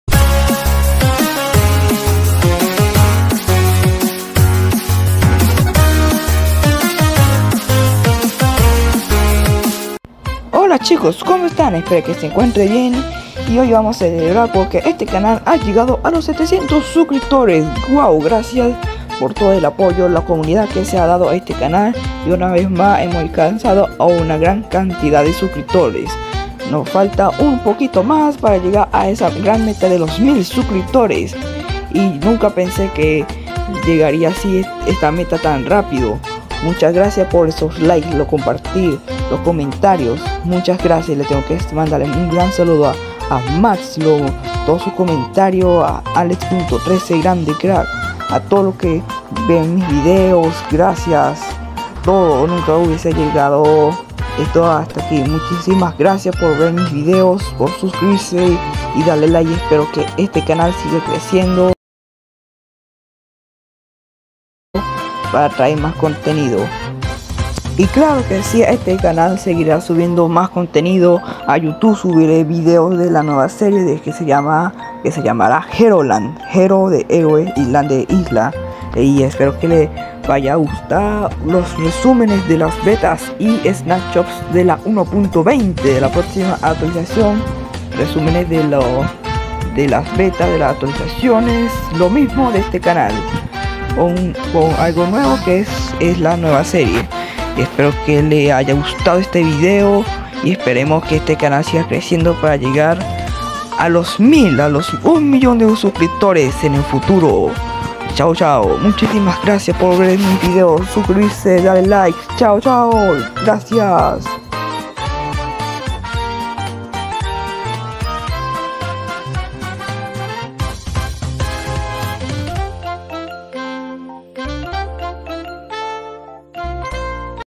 🎮👍Especial 700 Suscriptores👍🎮Agradecimientos:Alex.13,Maxlop,DiegoCM,SanbyCraft y Ranaa😀
Gracias por los 700 suscriptores nunca hubiese logrado este logro sin ustedes gracias ☺ :D